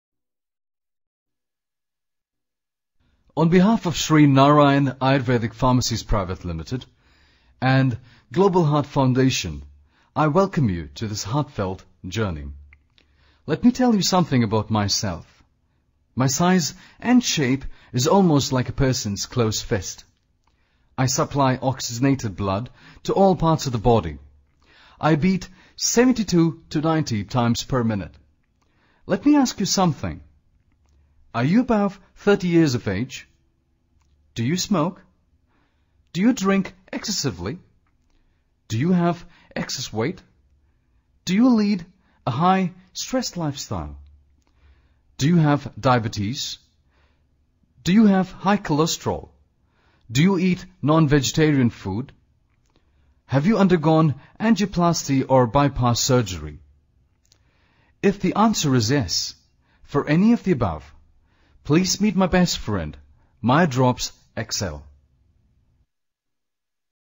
Global Heart Foundation Heart Treatment using natural way, specialised in non-invasive cardiology
EECP® Therapy – A simple, non-invasive solution to the complex problem of angina and heart failure.
EECP® Therapy is a safe, non-invasive, outpatient treatment option for patients suffering from ischemic heart diseases such as angina and heart failure.Global Heart Foundation being the pioneer, introduced EECP in Pune in 2009.